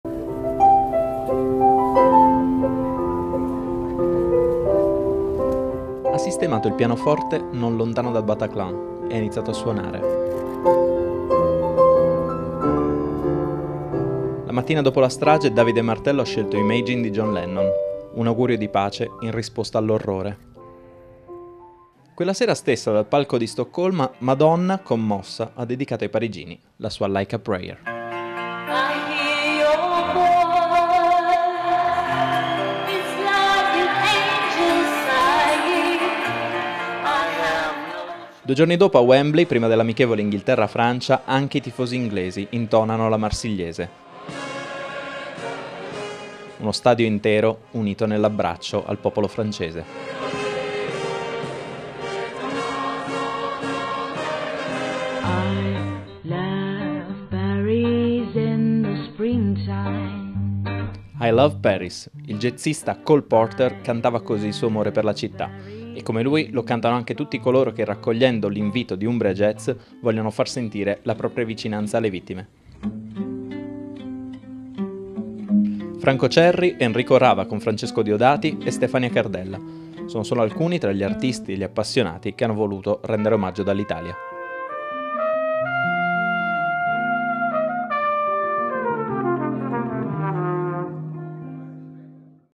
Parigi in musica
Servizio di Marco Frongia e Valentina Russo dal TG del 20 novembre 2015